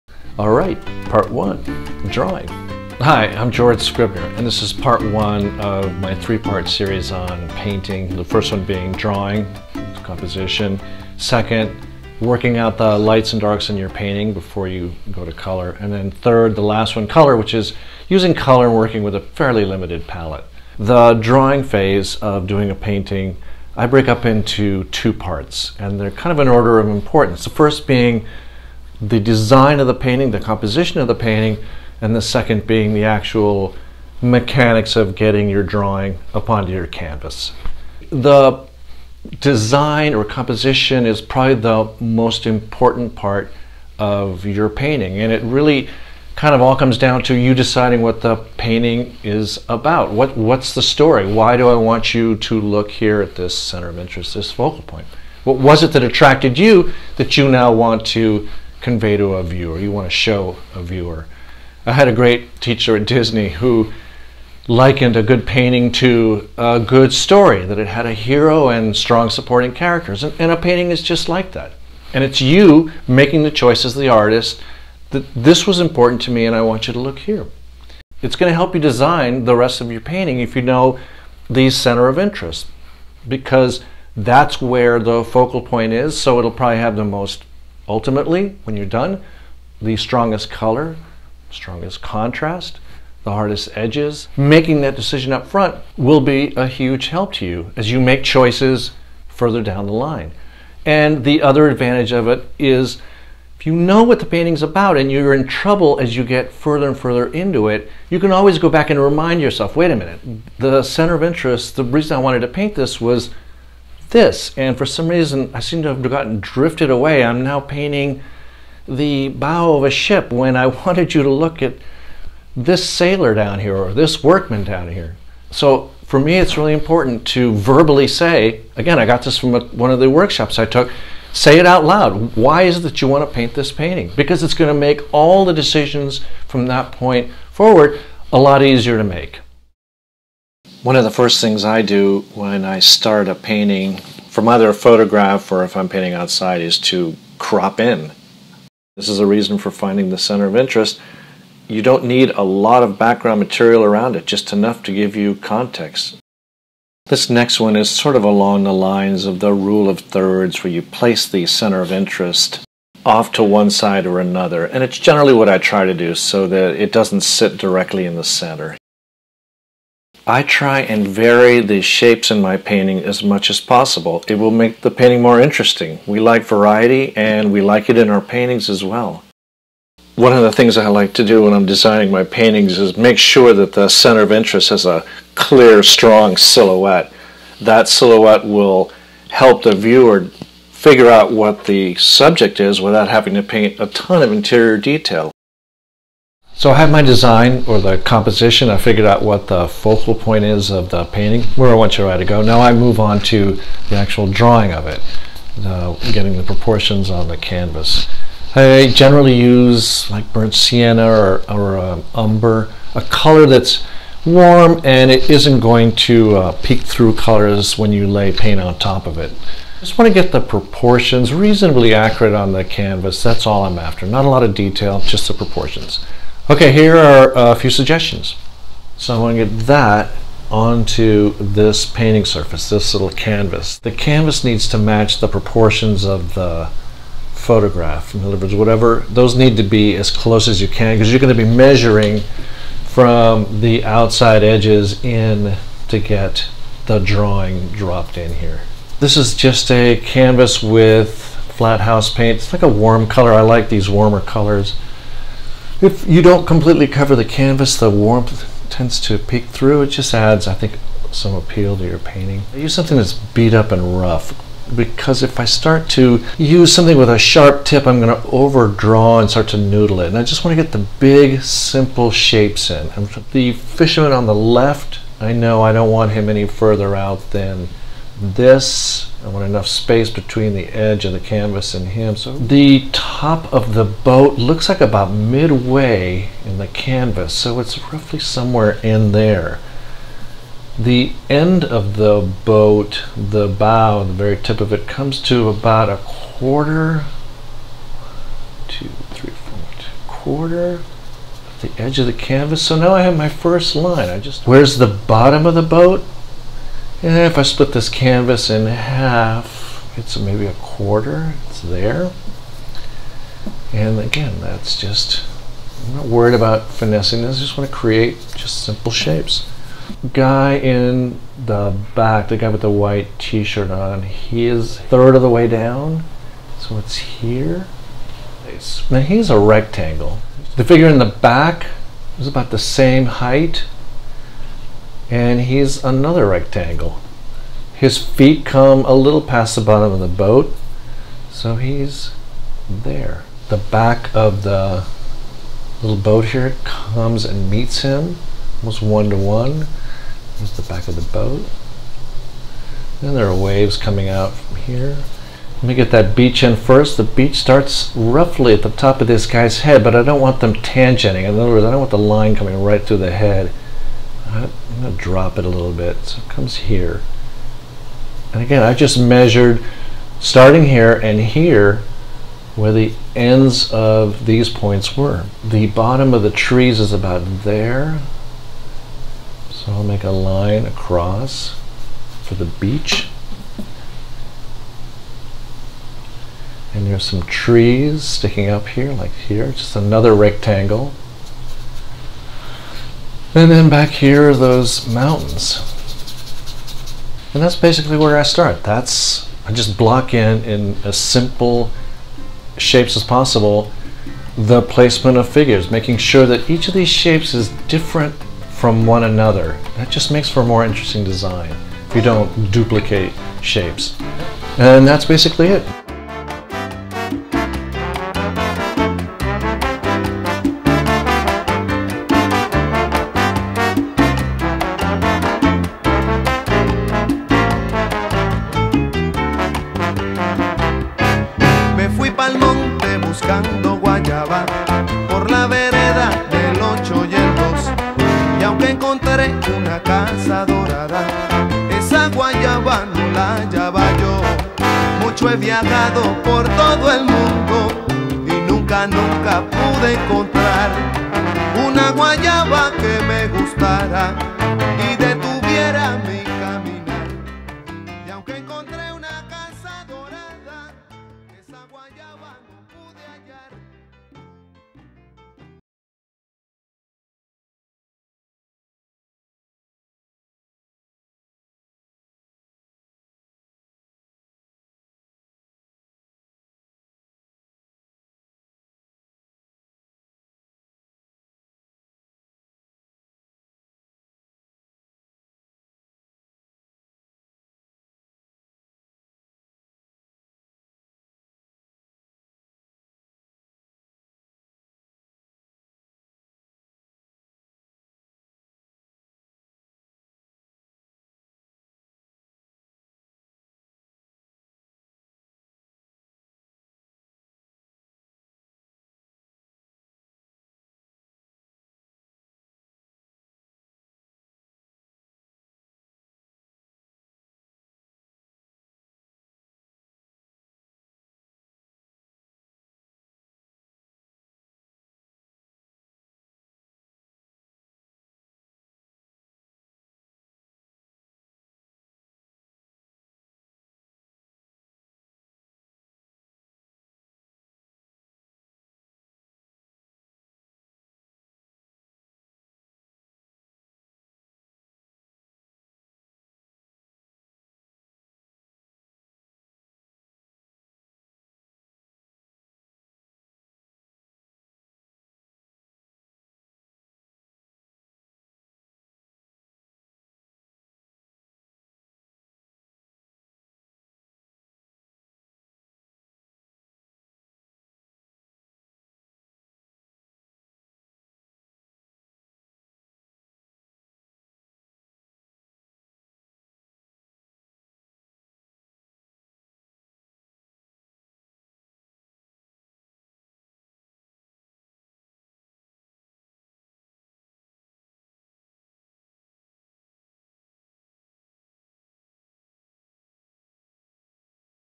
Part 1, a DRAWING Tutorial (or how I learned to love composition)
My first of three tutorials on Painting. These are a few suggestions for drawing and working out the design of your painting before you apply color. For those of you with more experience think of this as another approach to starting a painting. Enjoy!